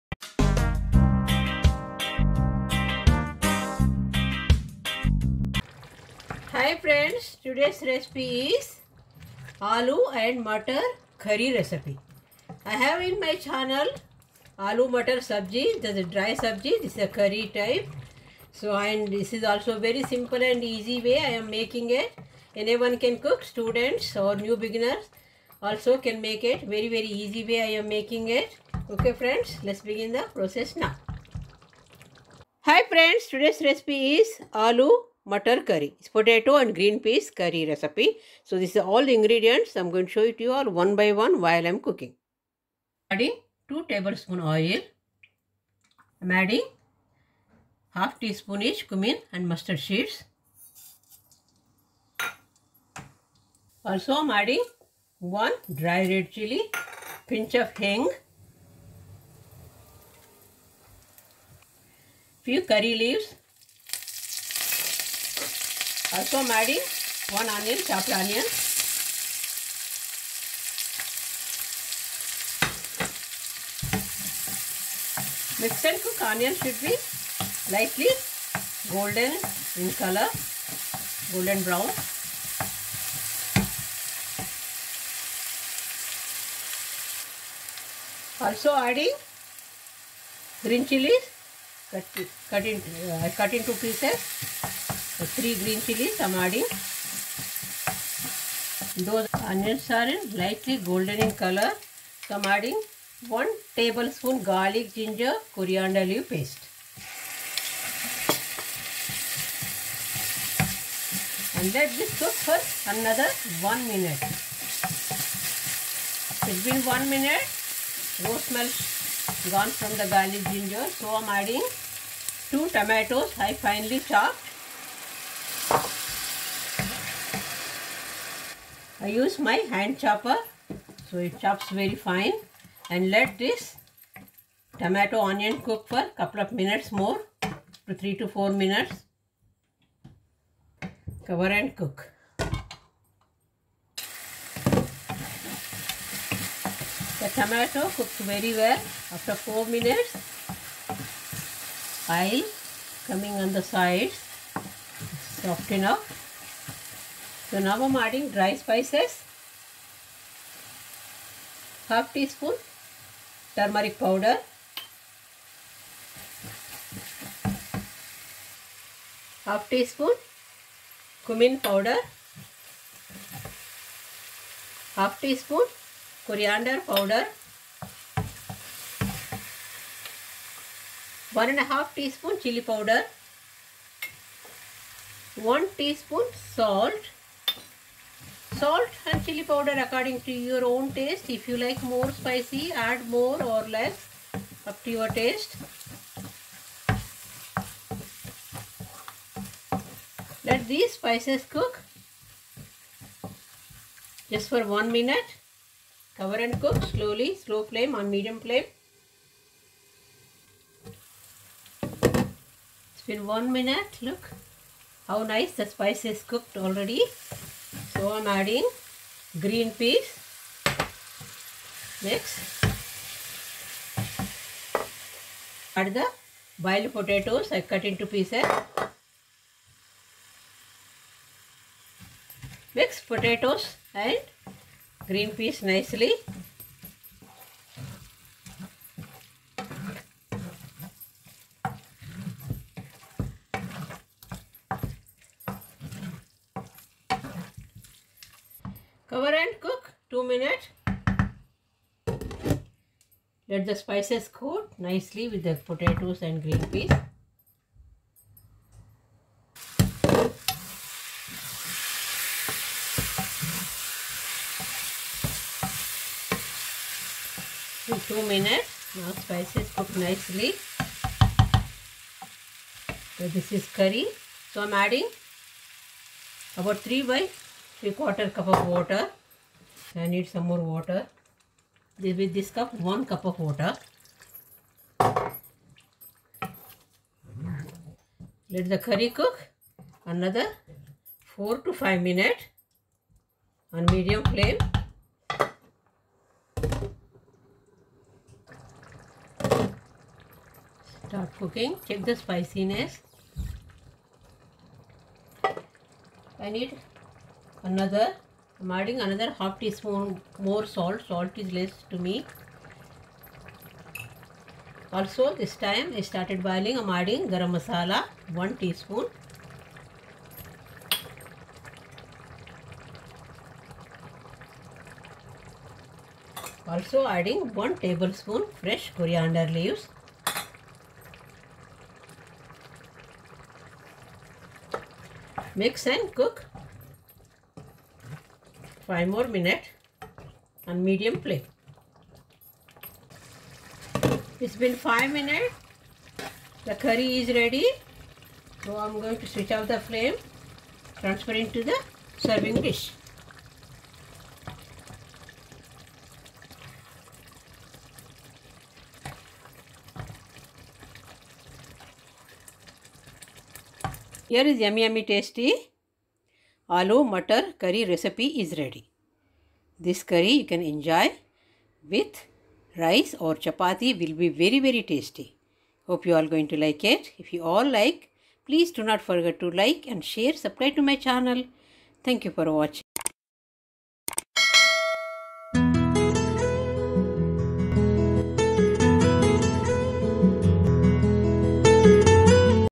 Aloo Matar Curry Recipe / Potato Green Peas Curry Recipe / Quick and Easy Recipe
Today's recipe is aloo matar curry, very simple and easy recipe. You can enjoy this curry with rice or roti.